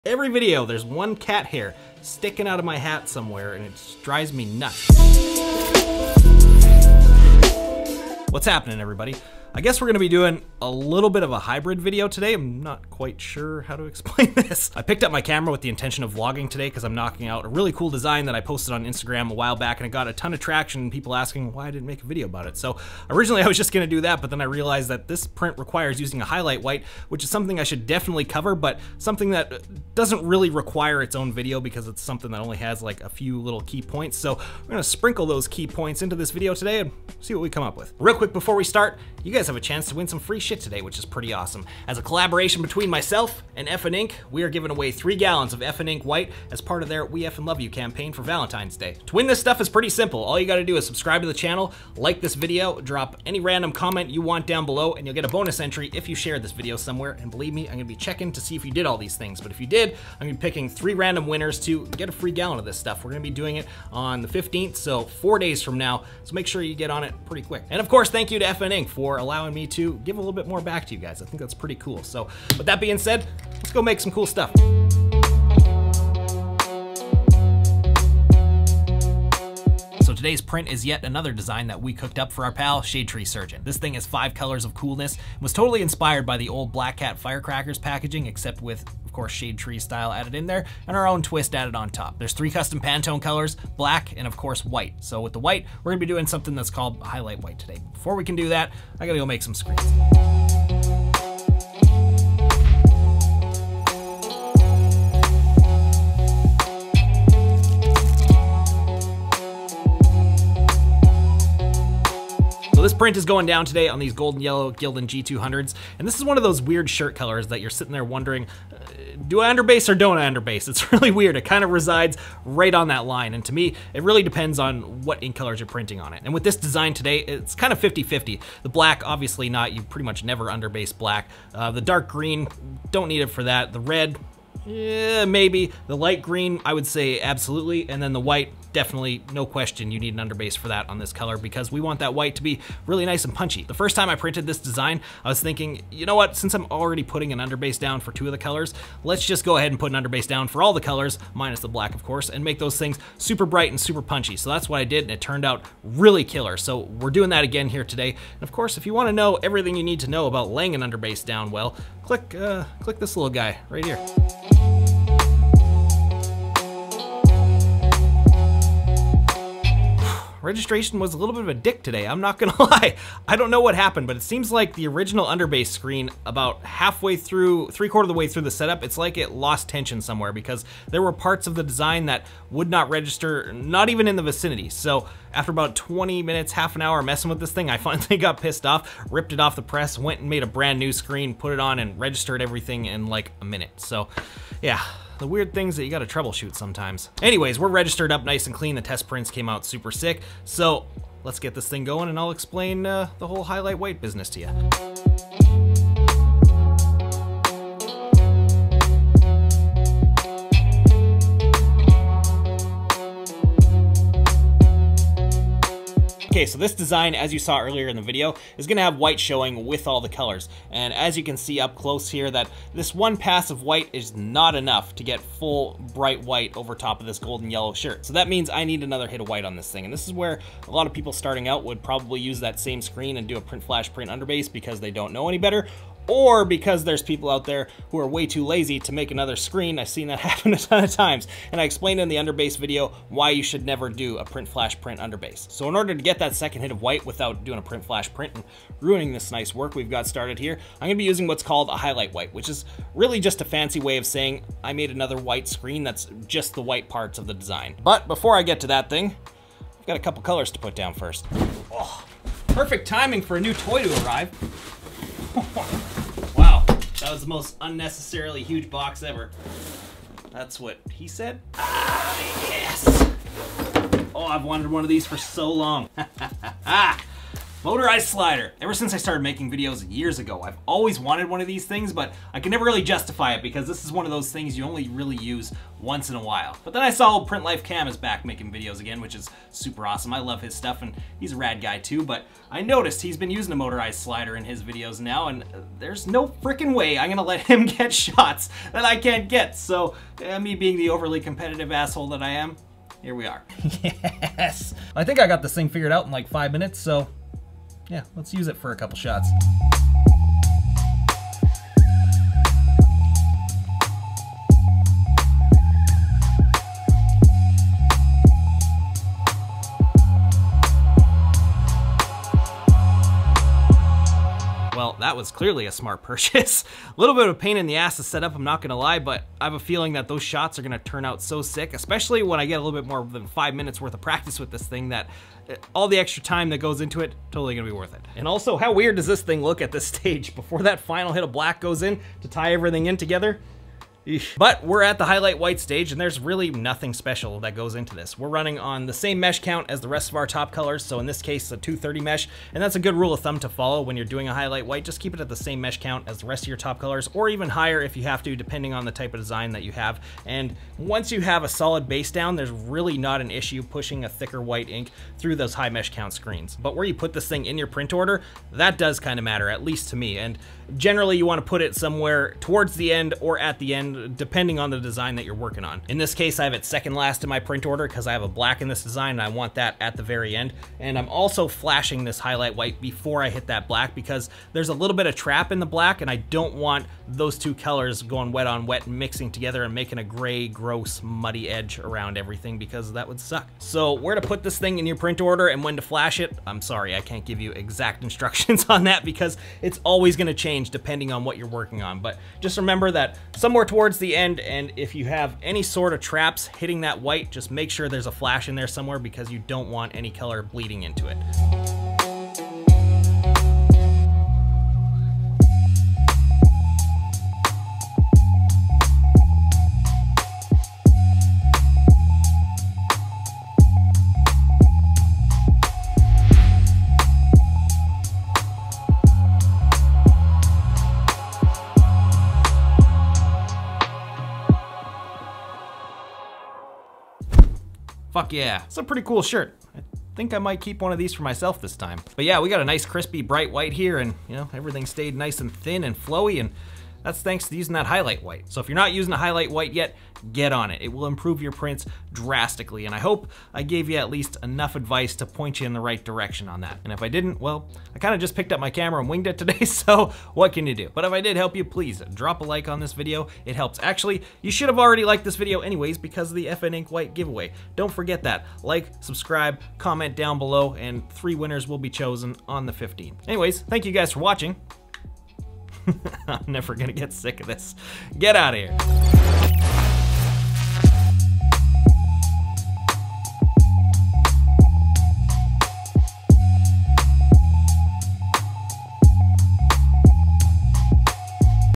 Using A Highlight White For Better Screen Prints
Screen print like a pro using a highlight white screen!

Interested In This Machine? Contact My Man Josh At Melco To Get The Hookup: